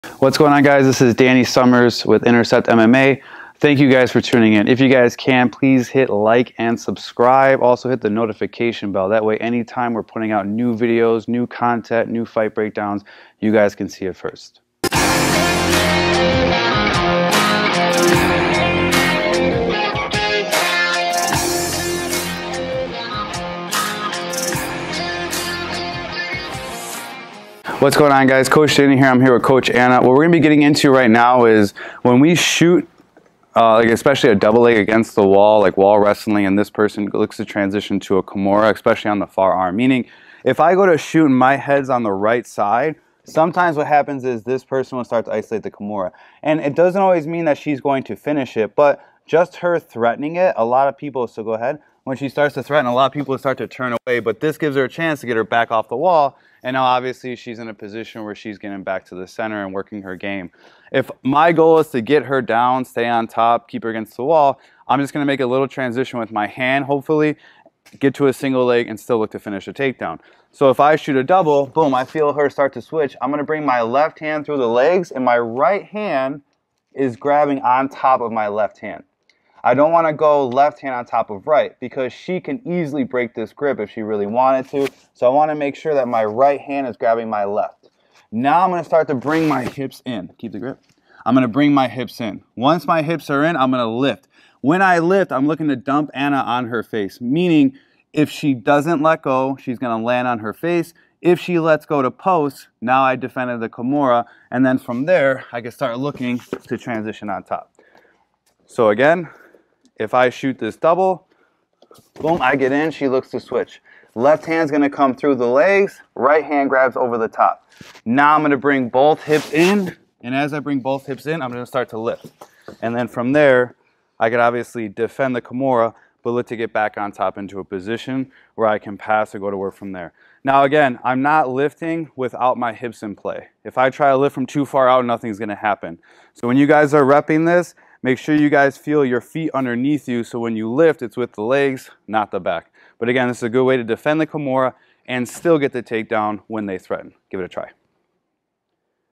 Defending the Kimura Against the Cage
Here we go over when someone uses a kimura attack when you are attempting to take them down against the wall/cage. This is an awesome way to not only defend the kimura but also still get the takedown. Give this one a try!